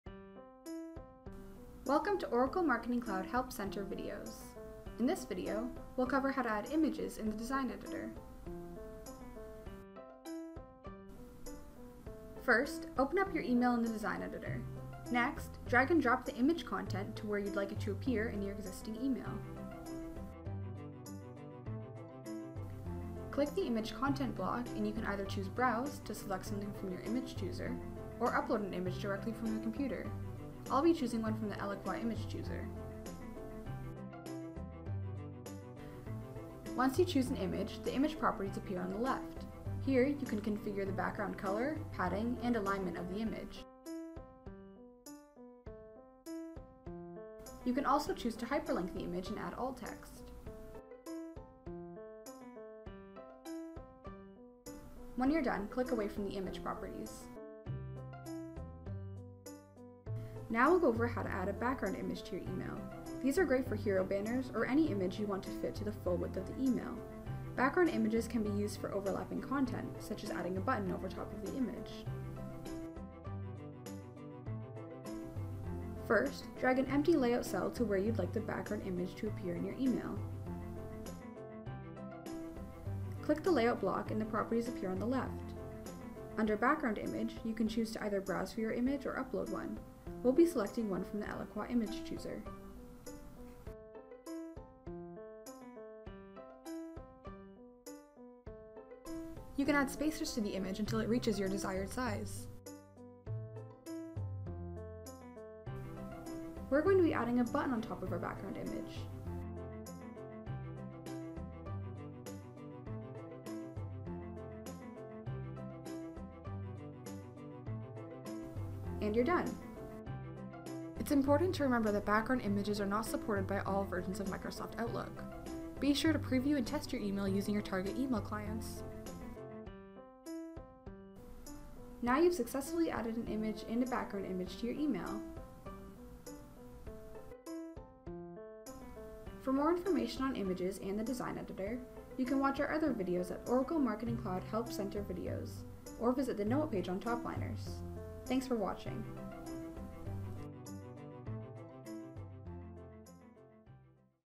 Oracle Eloqua - Adding Images in the Design Editor
Learn how to create compelling emails by adding images using Oracle Eloqua's Design Editor.